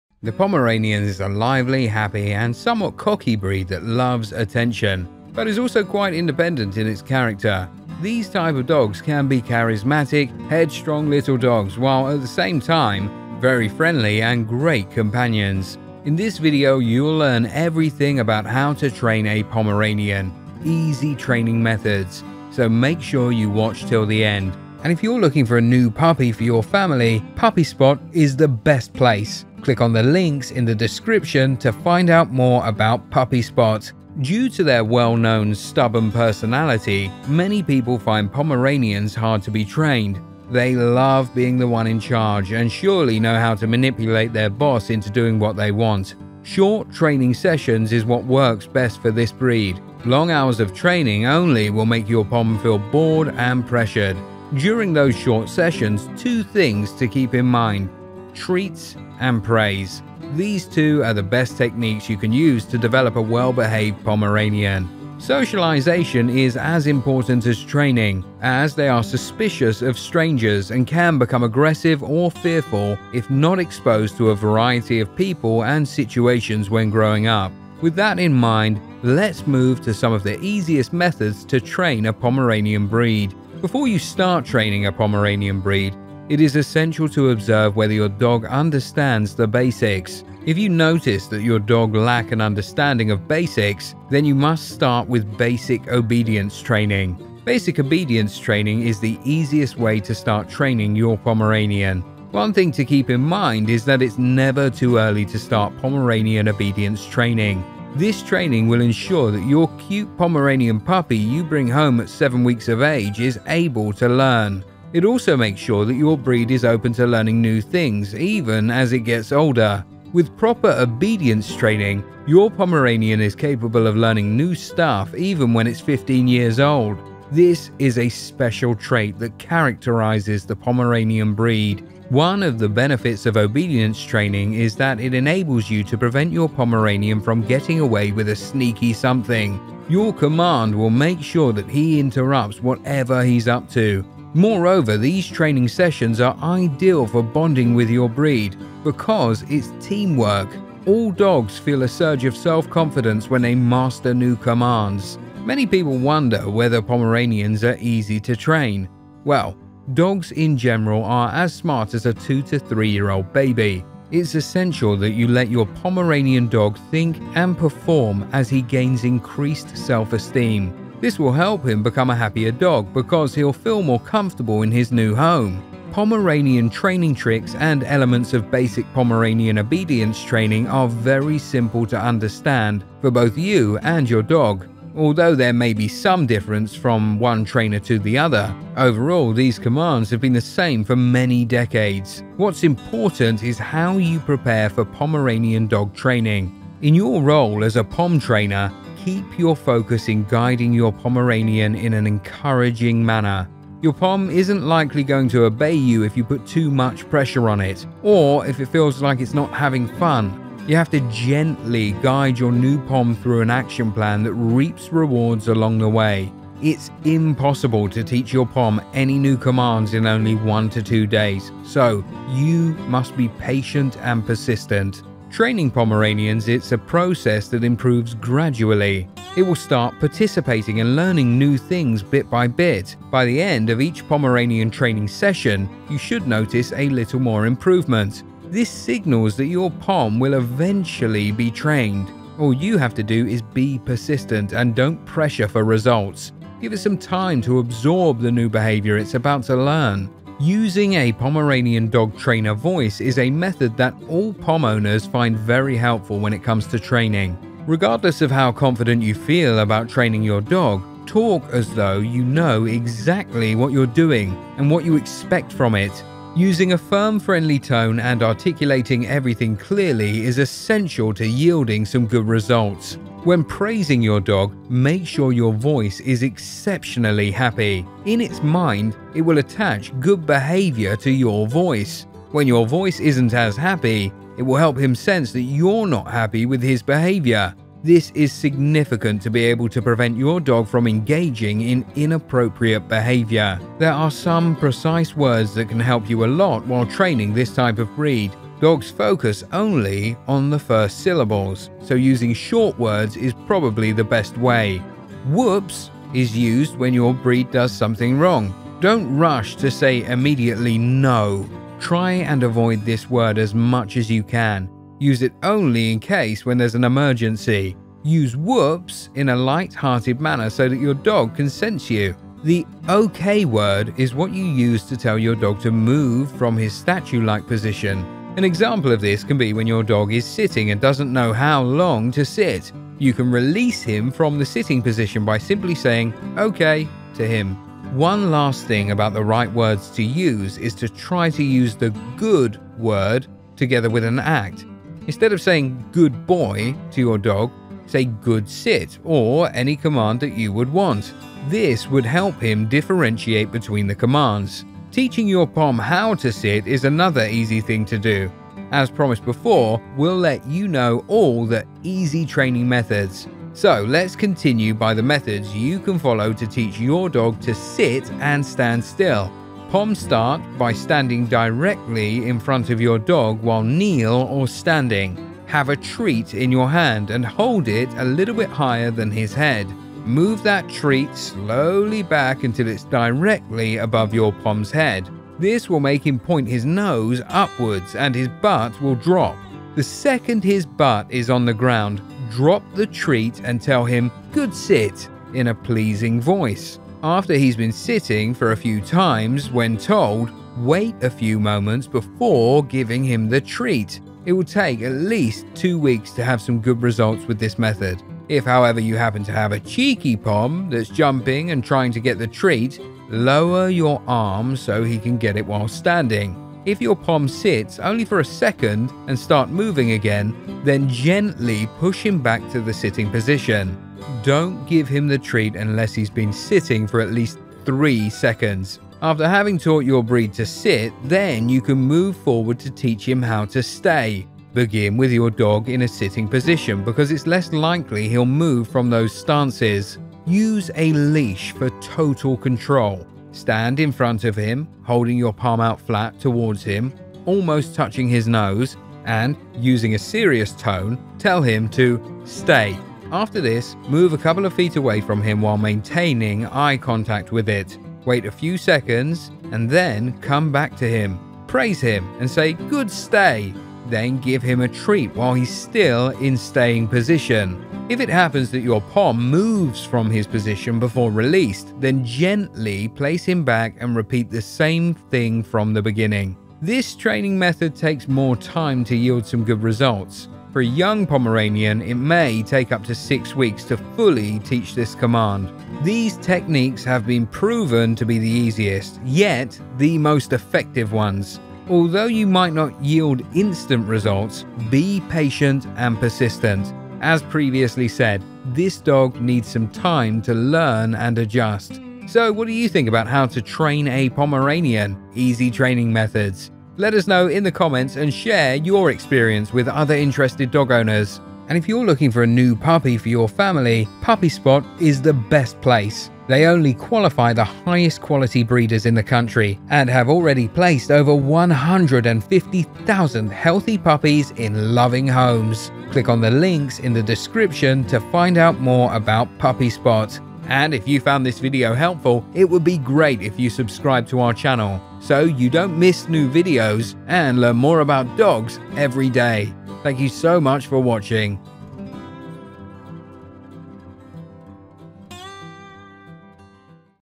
How To Train A Pomeranian | Dog World
Watch this video and learn how to train a Pomeranian dog.

The Pomeranian is a lively, happy and somewhat cocky breed that loves attention but is also quite independent in its character. These type of dogs can be charismatic, headstrong little dogs while at the same time very friendly and great companions
In this video you will learn everything about How To Train A Pomeranian  - Easy Training Methods, so make sure you watch till the end.
Also, check out the description for more helpful information and resources about dogs.

Due to their well-known stubborn-personality, many people find Pomeranians hard to be trained. They love being the one in charge and surely know how to manipulate their boss into doing what they want. Short training sessions is what works best for this breed. Long hours of training only will make your Pom feel bored and pressured. During those short sessions two things two keep in mind: Treats and praise. These two are the best techniques you can use to develop a well-behaved Pomeranian.
Socialization is as important as training, as they are suspicious of strangers and can become aggressive or fearful if not exposed to a variety of people and situations when growing up.

So what do you think about the How To Train A Pomeranian  - Easy Training Methods?